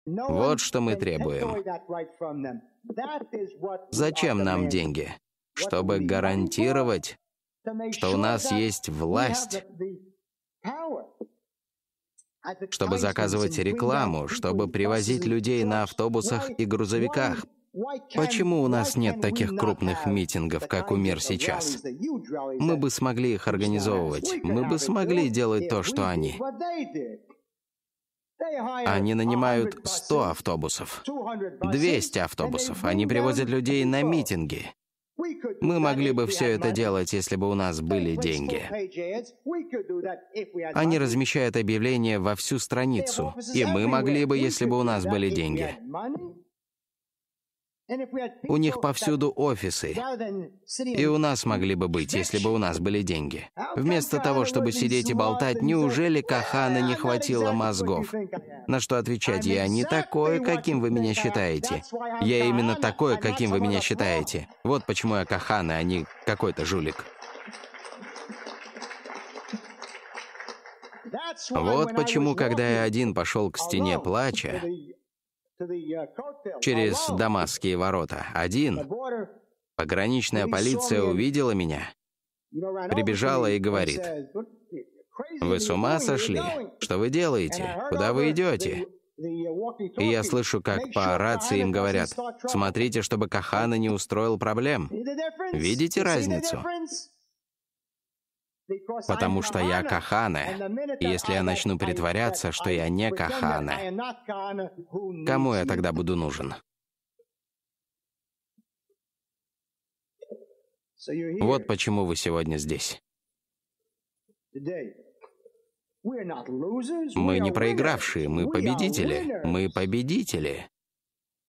12.15 Рабби Меир Кахане הי"ד. Потому, что я Кахане. Если я буду притворяться, что я не Кахане ...
- Потому, что я Кахане. Если я буду притворяться, что я не Кахане - кому я тогда буду нужен?! ... Я хочу спасти евреев от самих себя. Вы можете это сделать и вы обязаны это делать.